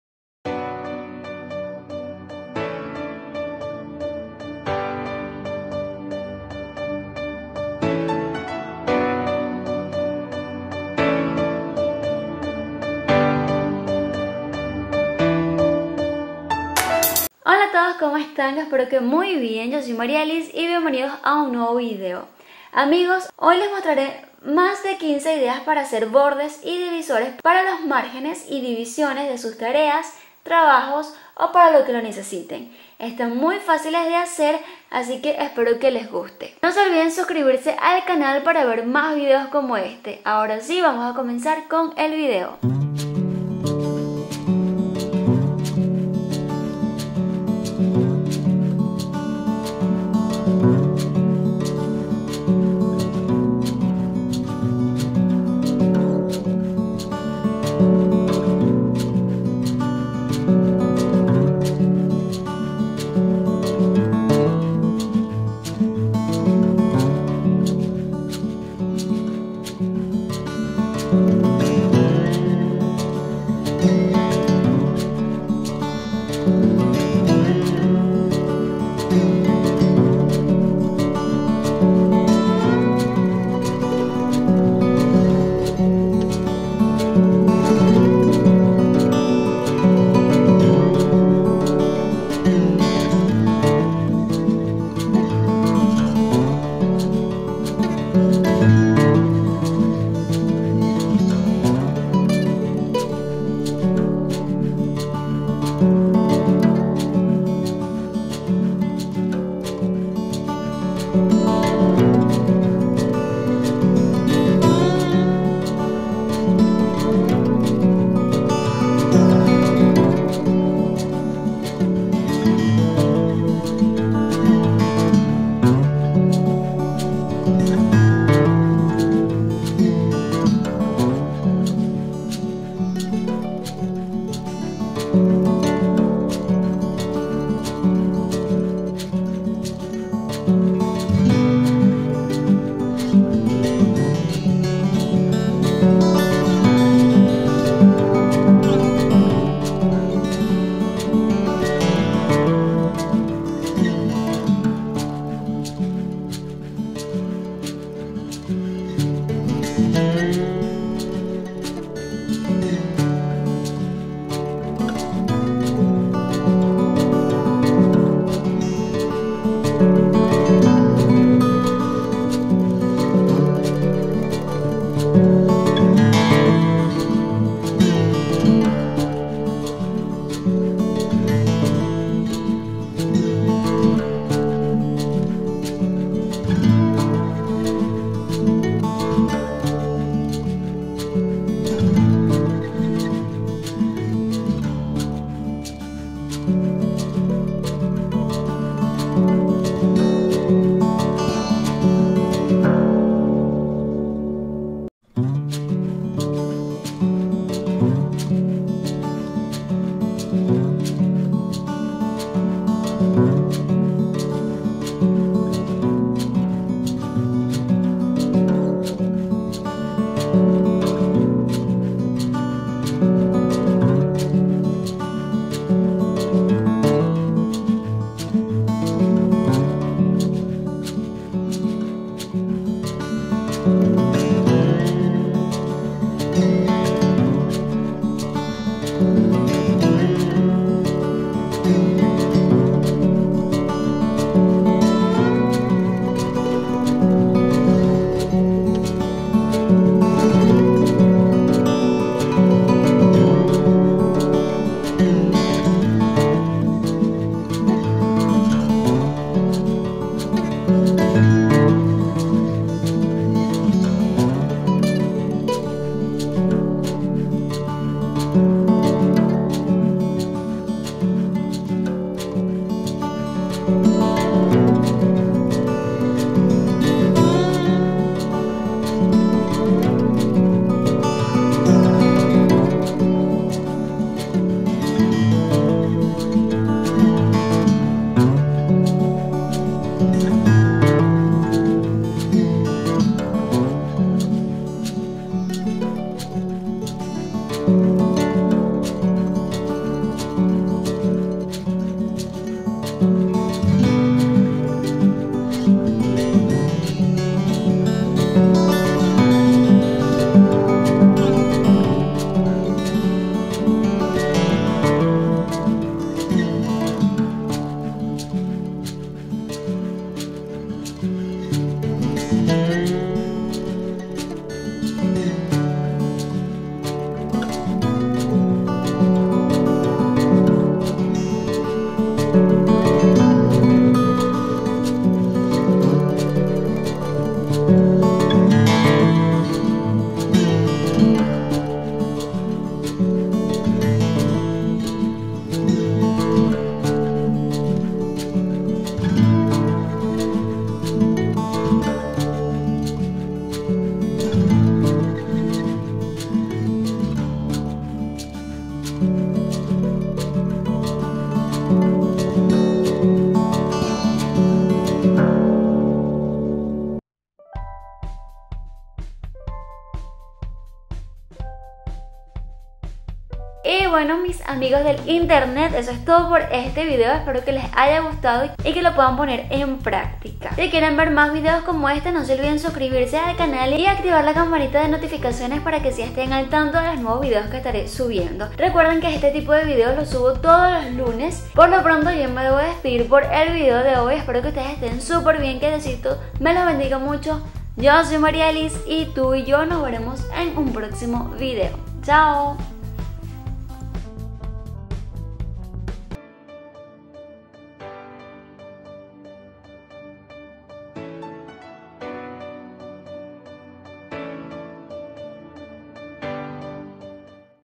BORDES, DIVISORES Y MÁRGENES PARA APUNTES BONITOS | Marialis ♥
Hola amigos! en el vídeo de hoy les muestro mas de 15 ideas para realizar márgenes, divisores o separadores para sus apuntes, trabajos, tareas o lo que necesiten.

Estas ideas están muy fáciles se hacer así que espero que se animen...

CONTACTO

QUE TENGAS UN LINDO DIA!